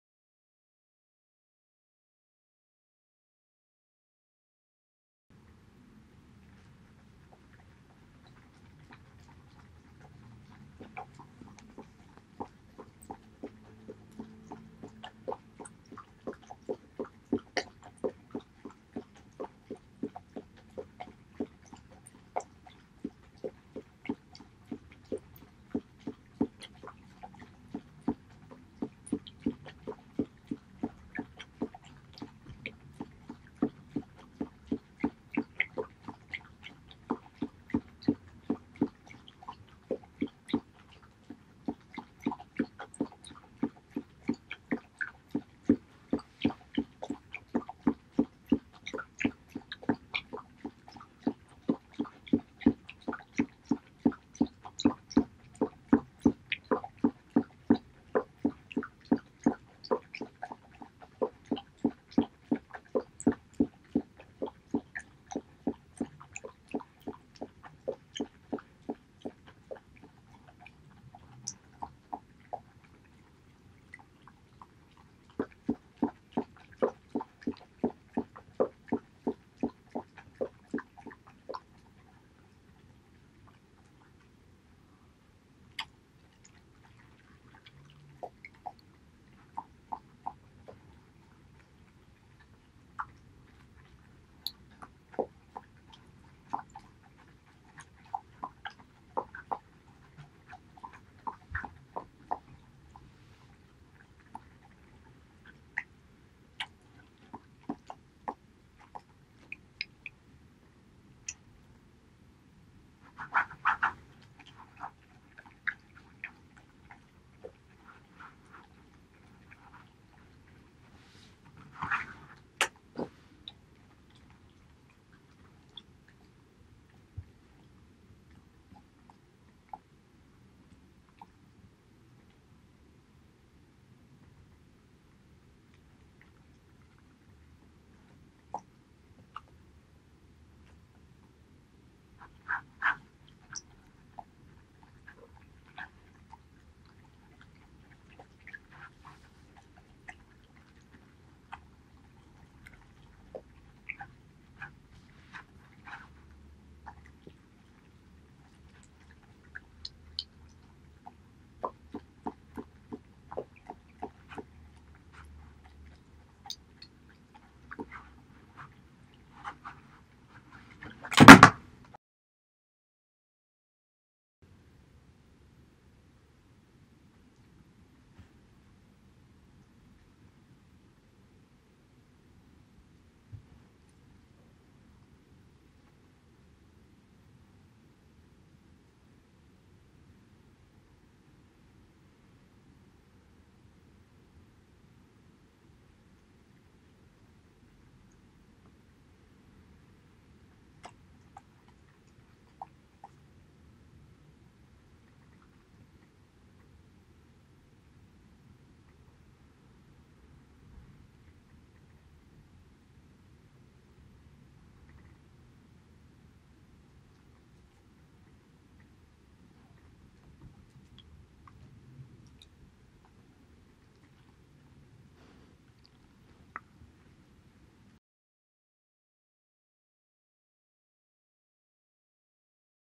ASMR Bordercollie eating pollack soup 보더콜리 황태죽 먹방
보더콜리 해든
Bordercolle Haedeun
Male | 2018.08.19